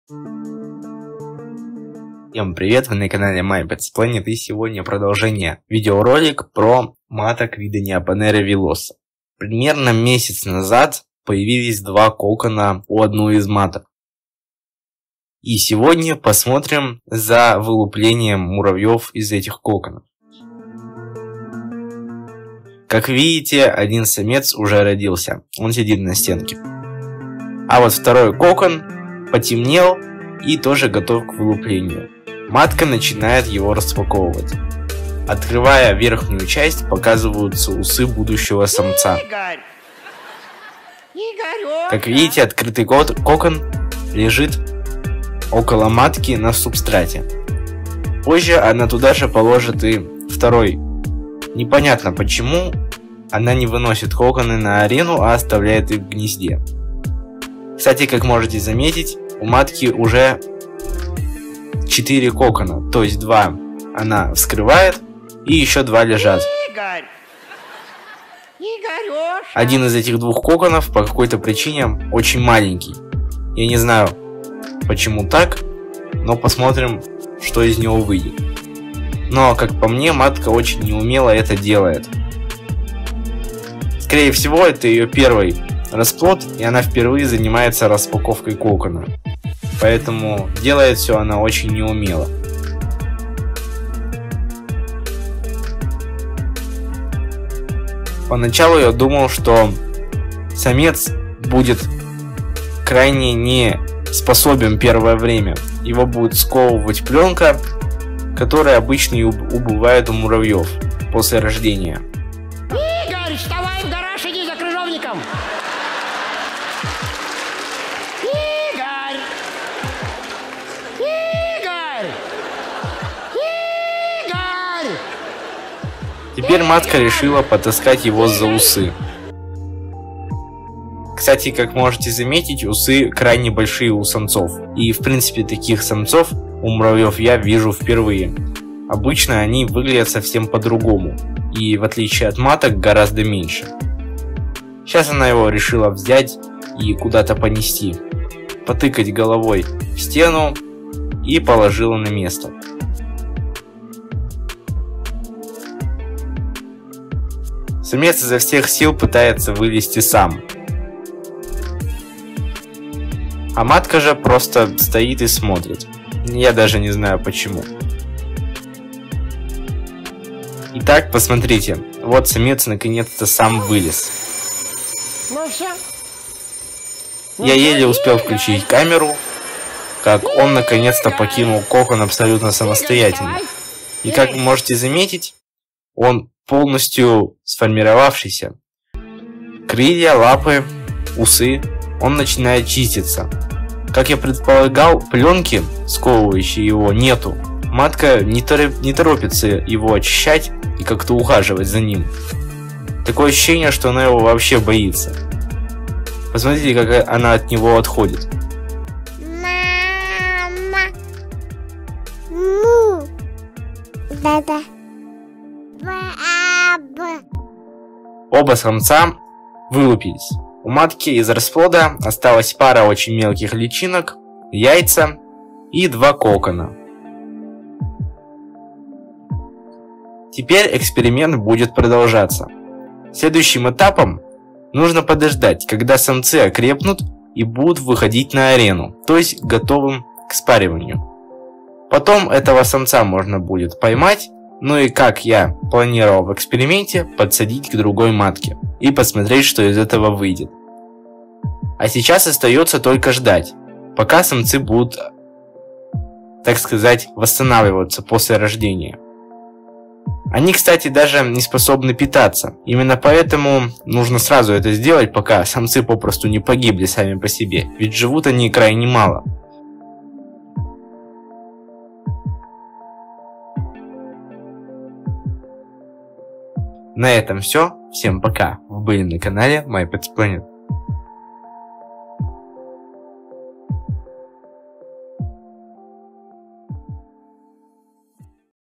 Как рождаются муравьи. Самец Neoponera villosa. Проект "Две матки"
В этом видео удалось заснять как рождается муравей самец вида Neoponera villosa. Как из кокона вылупляется большой и сразу с крыльями муравей самец.
Редкие кадры.